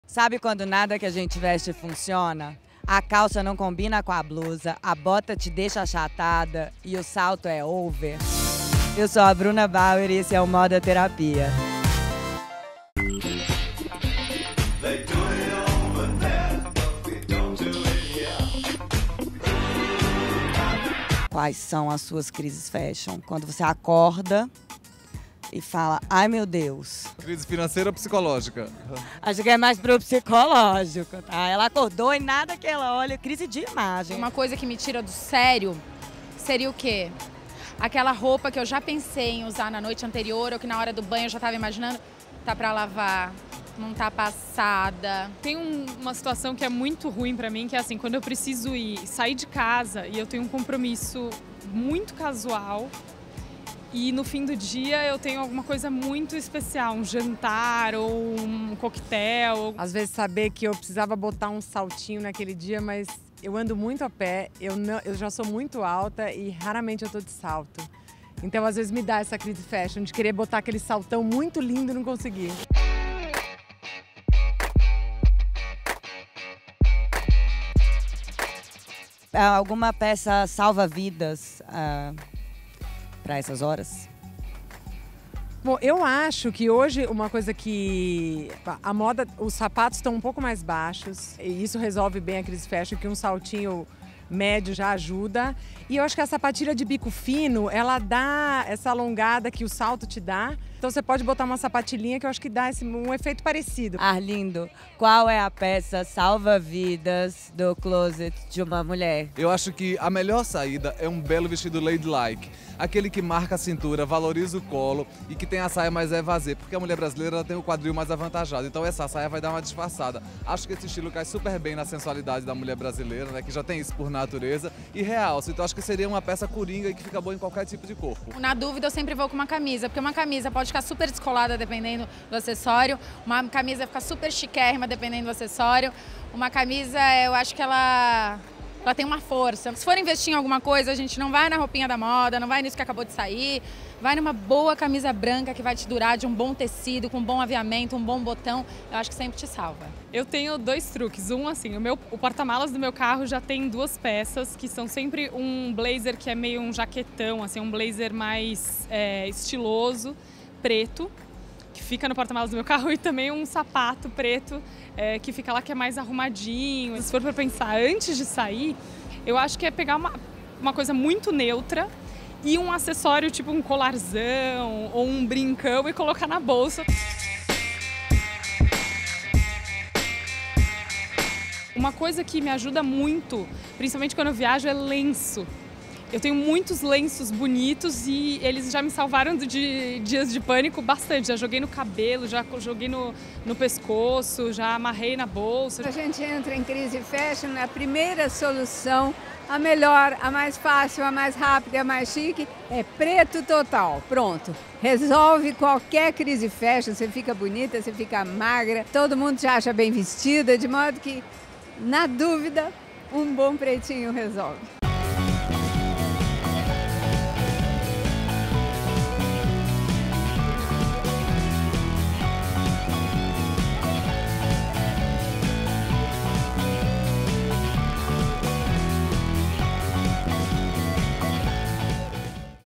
Crises fashion: as peças salva-vidas do guarda-roupa feminino
Sabe quando a gente acorda e nada que a gente veste funciona? Pois Bruna Bauer, nossa editora de moda, descobriu dicas incríveis para quem precisa resolver (e rápido!) esse tipo de problema. Vem ver!